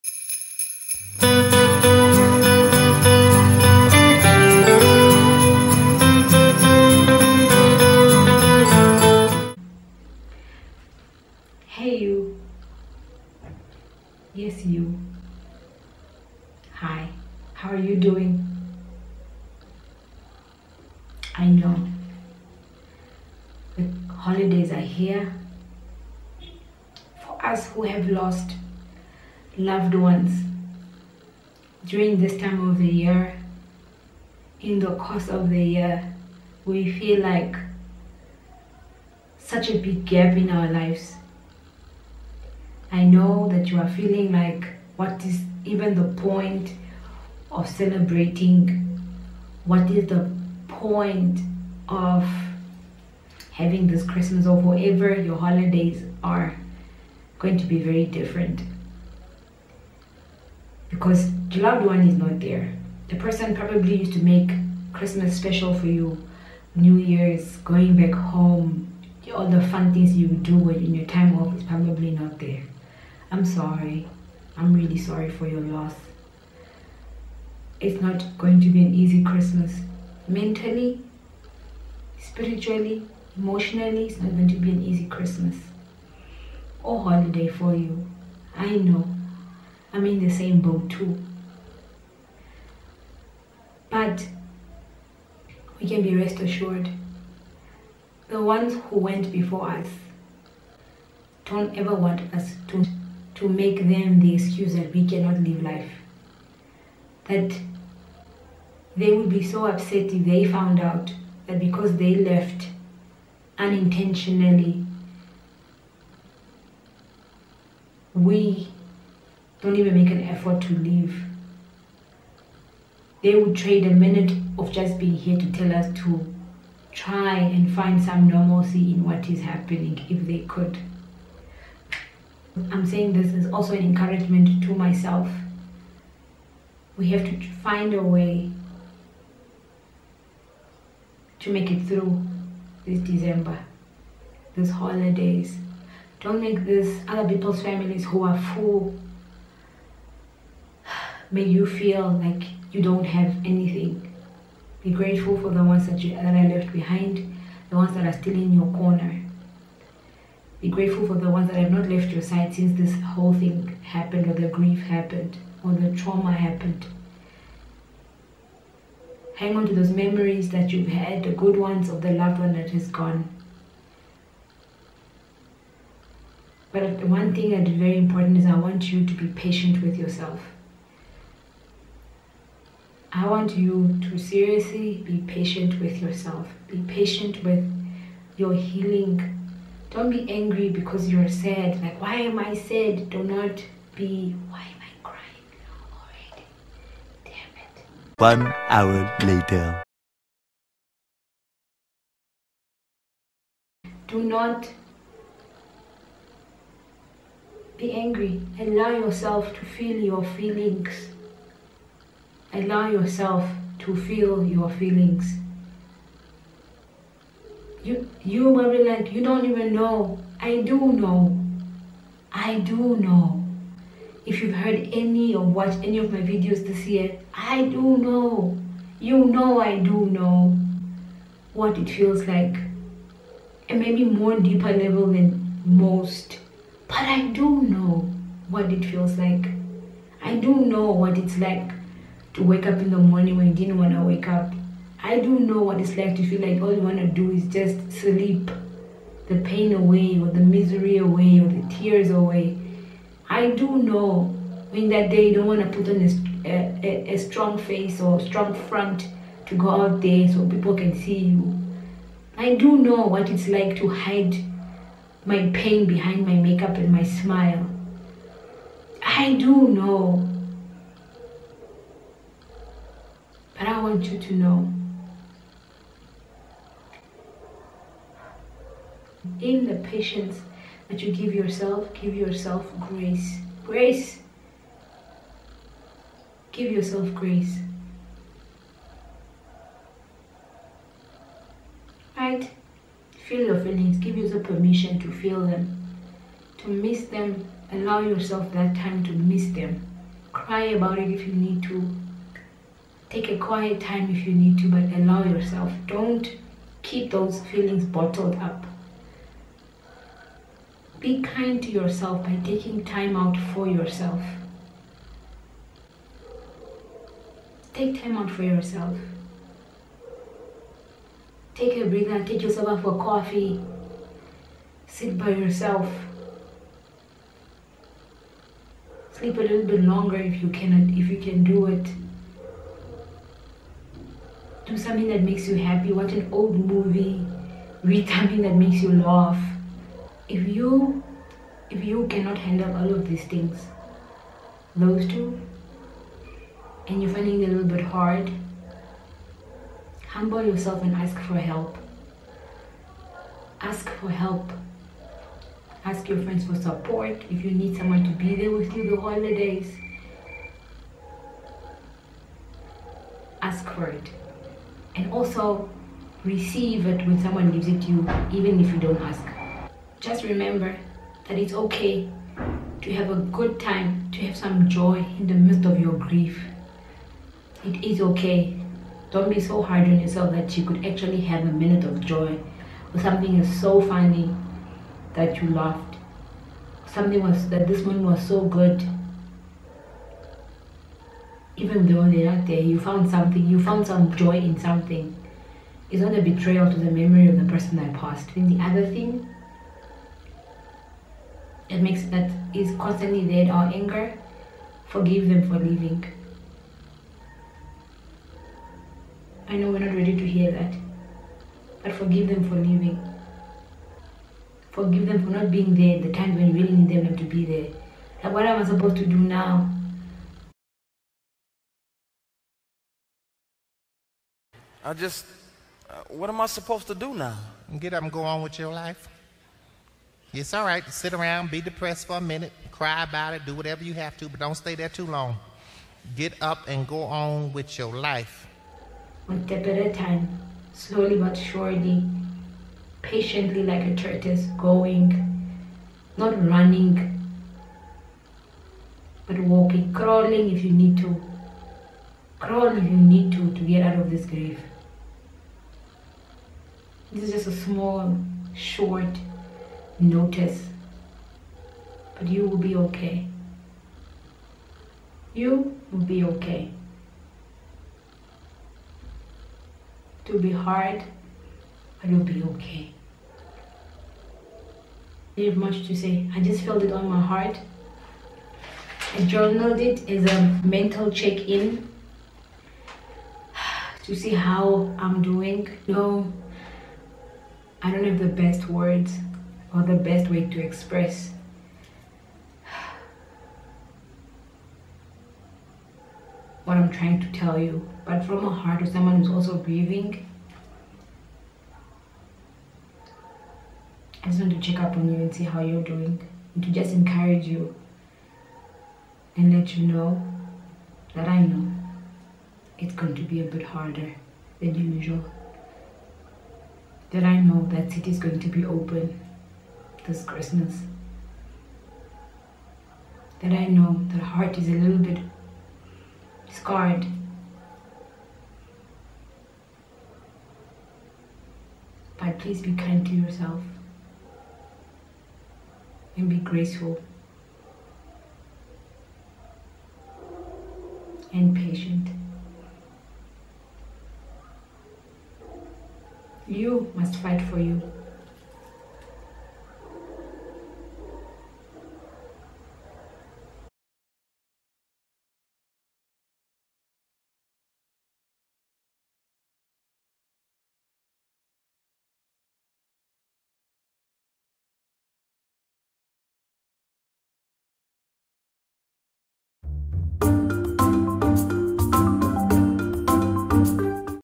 VLOGMAS DAY 19 | HEY YOU, YES YOU, COPING WITH GRIEF DURING THE HOLIDAY
Hey you, yes You, I know the holidays can be hard for those who are grieving.  I'm just checking in with a word of comfort.